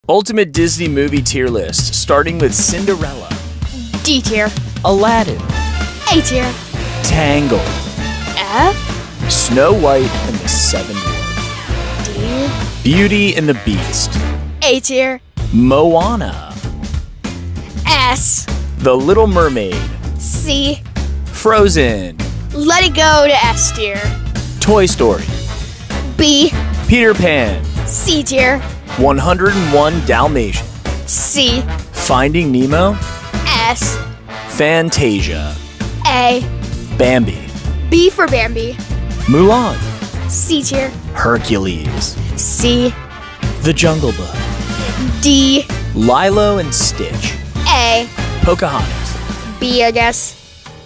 Ultimate Disney Movie Tier List 🎥🍿
Cinderella: A kind-hearted girl overcomes her stepmother's cruelty with the help of her fairy godmother and finds her true love at a magical ball.

Aladdin: A young "diamond in the rough" street thief discovers a magical lamp and with the help of a genie, embarks on a journey to win the heart of a princess and defeat an evil sorcerer.

Tangled: A spirited princess with magical long hair escapes from a tower, teams up with a charming bandit, and embarks on an adventure filled with humor, action, and romance.

Snow White and the Seven Dwarfs: A beautiful princess befriends seven dwarfs while hiding from her jealous stepmother, the wicked queen, who seeks to kill her.

Beauty and the Beast: A young woman finds herself falling in love with a cursed prince, trapped in the form of a beast, and must break the spell before a rose wilts and the prince remains a beast forever.

Finding Nemo: A clownfish embarks on a daring journey through the ocean to find his son, who has been captured by a scuba diver, encountering various sea creatures along the way.

The Little Mermaid: A curious mermaid princess dreams of becoming human and falls in love with a human prince, making a deal with a sea witch to trade her voice for legs.

Frozen: Two royal sisters, one with magical ice powers, must learn to embrace love and break a frozen curse to restore harmony to their kingdom.

Toy Story: A group of toys, led by Woody and Buzz Lightyear, come to life when humans aren't around and embark on adventures while navigating the dynamics of their changing relationships.

Peter Pan: A mischievous boy who never grows up takes three siblings to Neverland, where they encounter pirates, fairies, and face the ultimate showdown with the infamous Captain Hook.

101 Dalmatians: A kind-hearted couple's dalmatians puppies are kidnapped by the villainous Cruella de Vil, and with the help of other dogs, they work together to rescue their adorable pups.

Fantasia: A collection of animated musical segments set to classical music, featuring colorful and imaginative visuals that bring the music to life.

Bambi: A young deer named Bambi navigates the trials and joys of the forest with his friends Thumper and Flower, while learning about love, loss, and the circle of life.

Mulan: A courageous young woman disguises herself as a man to take her father's place in the Chinese army, fighting against invaders and becoming a legendary hero.

Hercules: A Greek demigod, Hercules, embarks on a journey to become a true hero, facing challenges, defeating mythical creatures, and discovering his own strength.

The Jungle Book: A young boy named Mowgli, raised by wolves in the jungle, embarks on a journey to find his place among humans, encountering friendly and treacherous animals along the way.

Lilo and Stitch: A young Hawaiian girl named Lilo adopts an alien creature named Stitch, who initially causes chaos but ultimately learns the value of family and friendship.

Pocahontas: A Native American princess, Pocahontas, falls in love with an English settler, John Smith, amidst the conflict between their cultures, teaching both sides the importance of understanding and respect.